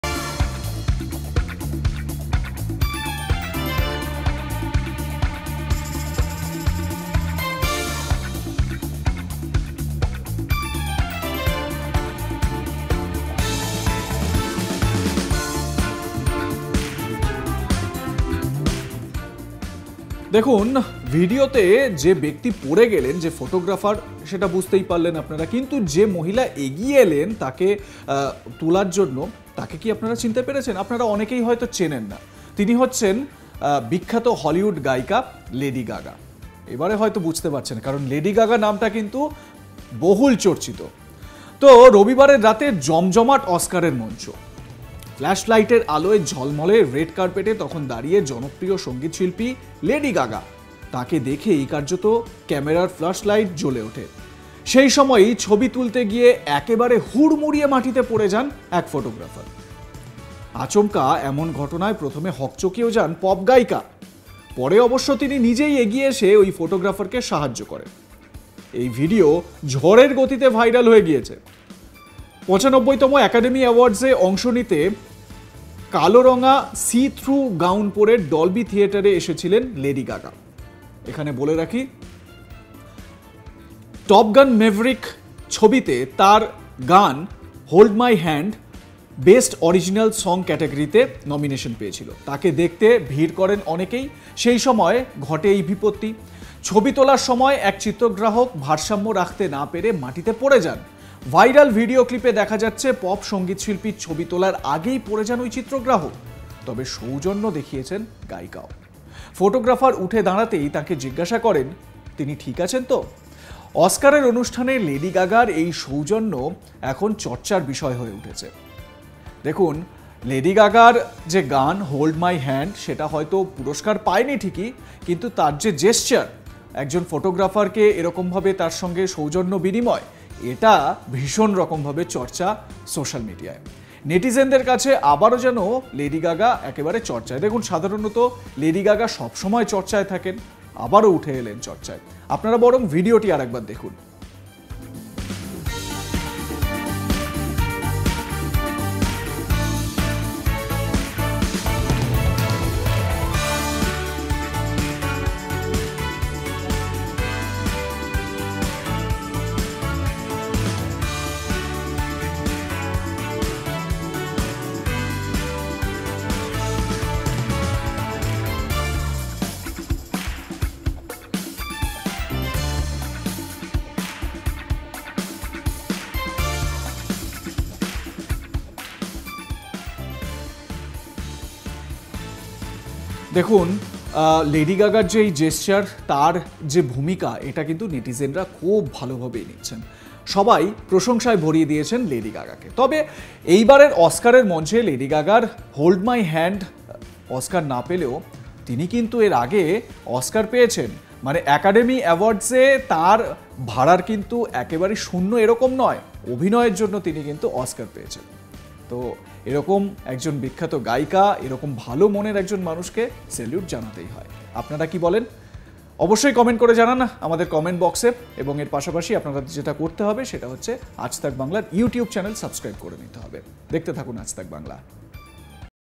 লেডি গাগার সামনেই মুখ থুবড়ে পড়লেন ফটোগ্রাফার! Lady Gaga | Oscar Awards 2023 | Academy Awards
রবিবারের রাতে জমজমাট অস্কার মঞ্চ। ফ্ল্যাশলাইটের আলোয় ঝলমলে রেড কার্পেটে তখন দাঁড়িয়ে জনপ্রিয় সঙ্গীতশিল্পী লেডি গাগা। তাকে দেখেই কার্যত ক্যামেরার ফ্ল্যাস লাইট জ্বলে ওঠে। সেই সময়ই ছবি তুলতে গিয়ে একেবারে হুড়মুড়িয়ে মাটিতে পড়ে গেলেন এক ফটোগ্রাফার। আচমকা এমন ঘটনায় প্রথমে হকচকিয়েও যান পপ গায়িকা। পরে অবশ্য তিনি নিজেই এগিয়ে এসে ওই ফটোগ্রাফারকে সাহায্য করেন। এই ভিডিও ঝড়ের গতিতে ভাইরাল হয়ে গিয়েছে। 95 তম অ্যাকাডেমি অ্যাওয়ার্ডসে অংশ নিতে কালোরঙা সি-থ্রু গাউন পরে ডলবি থিয়েটারে এসেছিলেন লেডি গাগা। তাকে দেখতে ভিড় করেন অনেকেই। সেই সময় ঘটে এই বিপত্তি। ছবি তোলার সময় এক চিত্রগ্রাহক ভারসাম্য রাখতে না পেরে মাটিতে পড়ে যান। ভাইরাল ভিডিও ক্লিপে দেখা যাচ্ছে, পপ সঙ্গীতশিল্পীর ছবি তোলার আগেই পড়ে যান ওই চিত্রগ্রাহক। তবে সৌজন্যও দেখিয়েছেন গায়িকাও। ফটোগ্রাফার উঠে দাঁড়াতেই তাঁকে জিজ্ঞেস করেন, তিনি ঠিক আছেন কি না! অস্কারের অনুষ্ঠানে লেডি গাগার এই সৌজন্যে এখন চর্চার বিষয় হয়ে উঠেছে। নেটিজেনদের কাছে আবারও যেন চর্চায় লেডি গাগা।